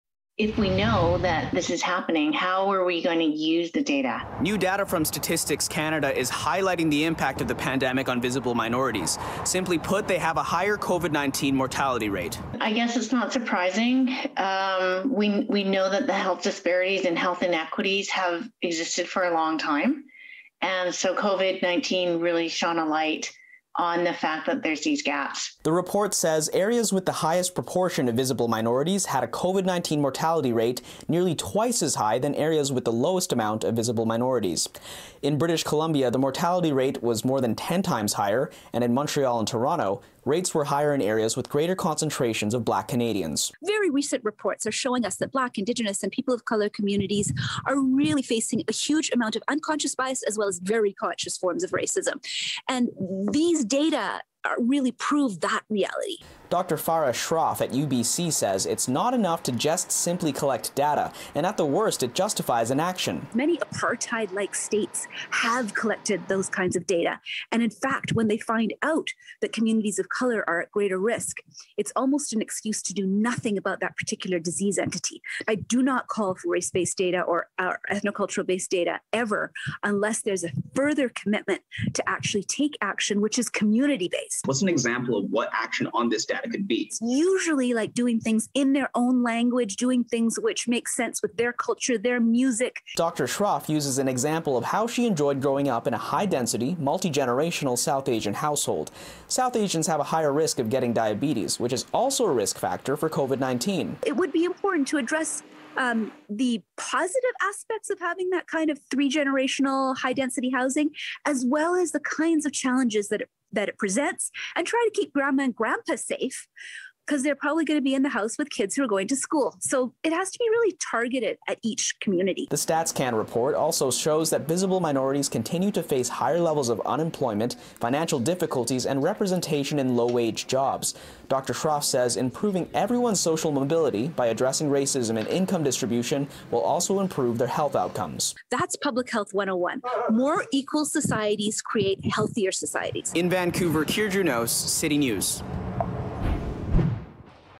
New StatsCan report shows higher COVID-19 mortality rate for visible minorities
For some time, experts have raised concerns about the uneven health impacts of the pandemic on visible minorities. As Kier Junos reports, new StatsCan data highlights just how severe those impacts are.